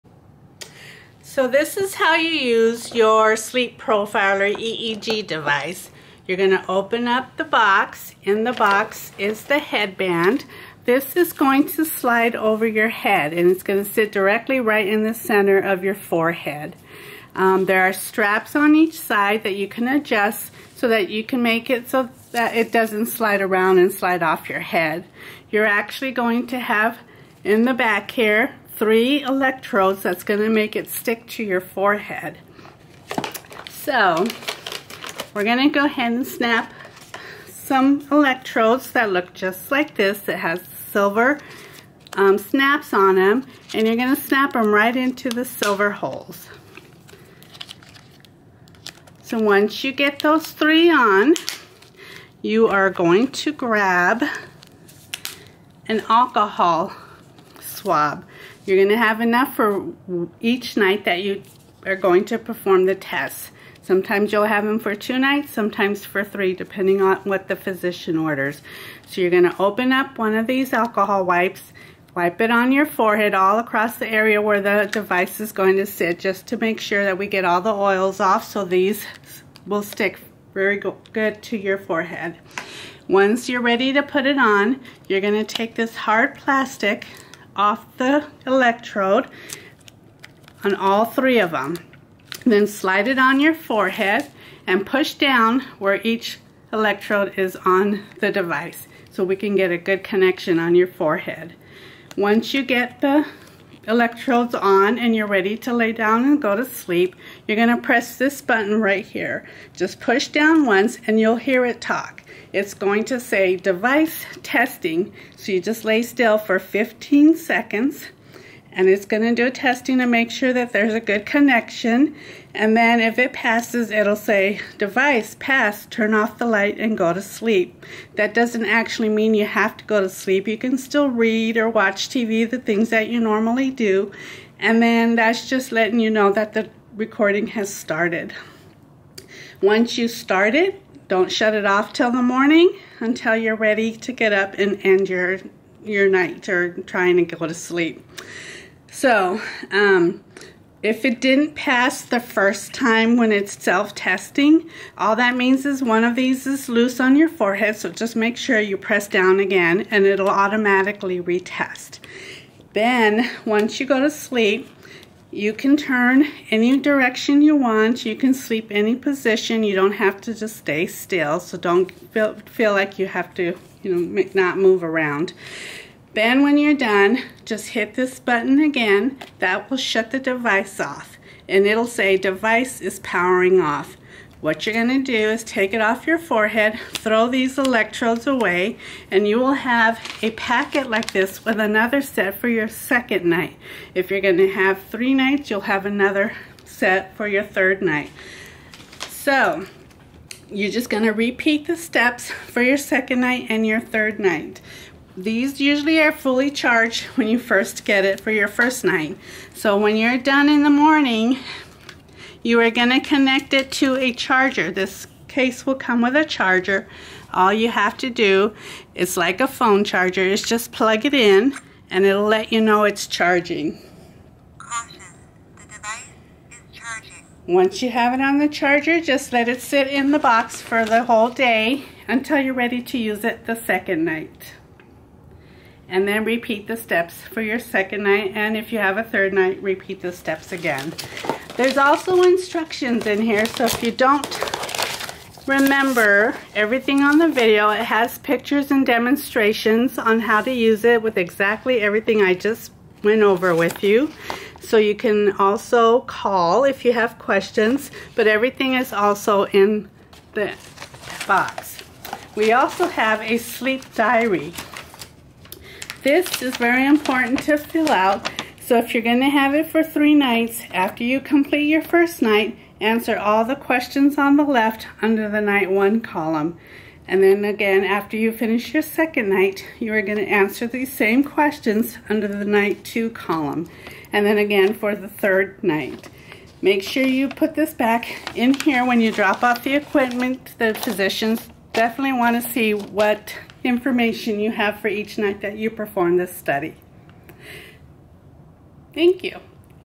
Sleep Profiler Setup / The Insomnia and Sleep Institute of Arizona
This video reviews how to setup the Sleep Profiler unit for your at home insomnia testing.